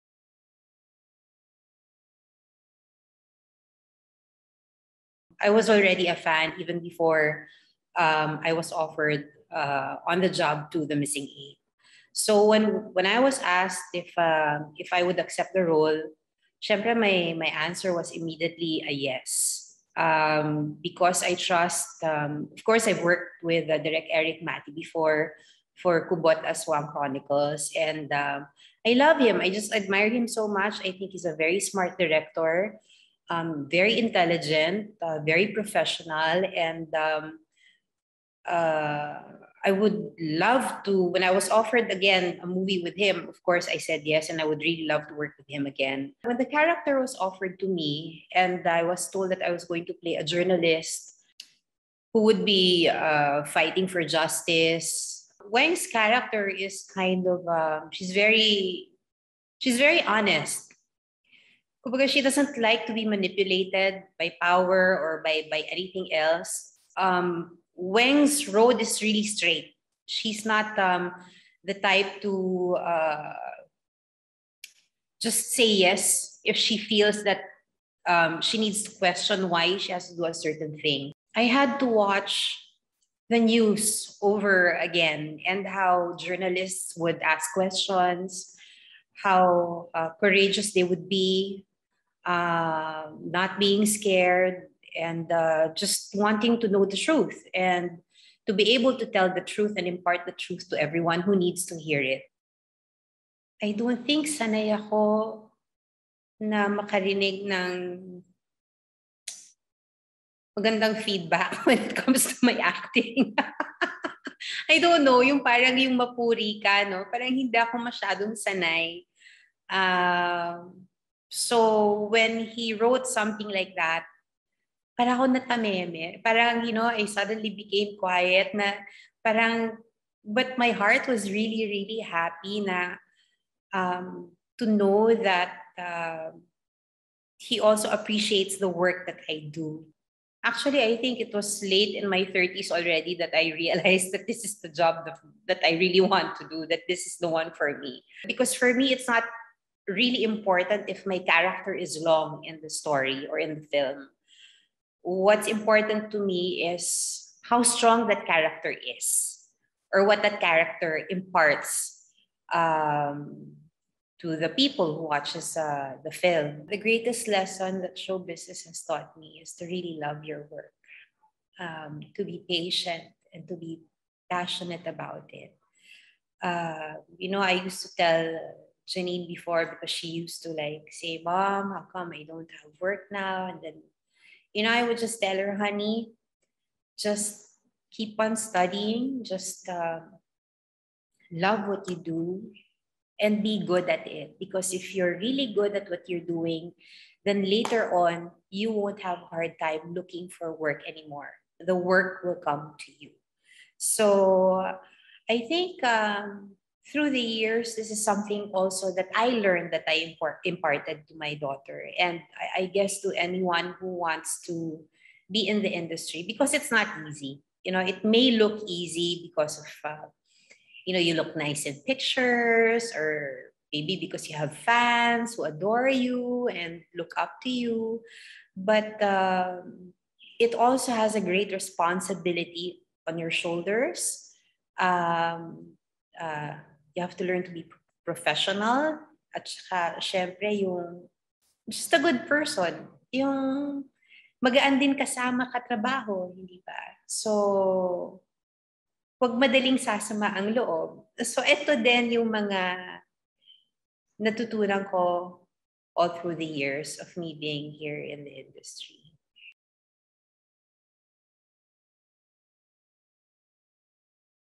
Lotlot De Leon on why immediately she said yes to “On the Job: The Missing 8”
Lotlot De Leon won the Best Supporting Actress award at the 2014 Manilla Film Festival in her first film with director Erik Matti, “Kubot: The Aswang Chronicles 2.” When she was offered the part in “On the Job: The Missing 8,” she immediately said yes.

“I was told that I was going to play a journalist who would be fighting for justice,” she says. “Weng’s road is really straight. She’s not the type to say yes if she feels that she needs to question why she has to do a certain thing. I have to watch the news over again and how the journalists would ask questions, how courageous they would be, not being scared and just wanting to know the truth and impart the truth to everyone who needs to hear it.”

Here she talks more about her role in the acclaimed film and why it doesn’t matter whether her character is prominent in the film she’s starring in.

Interview and video by SAMANTHA LEE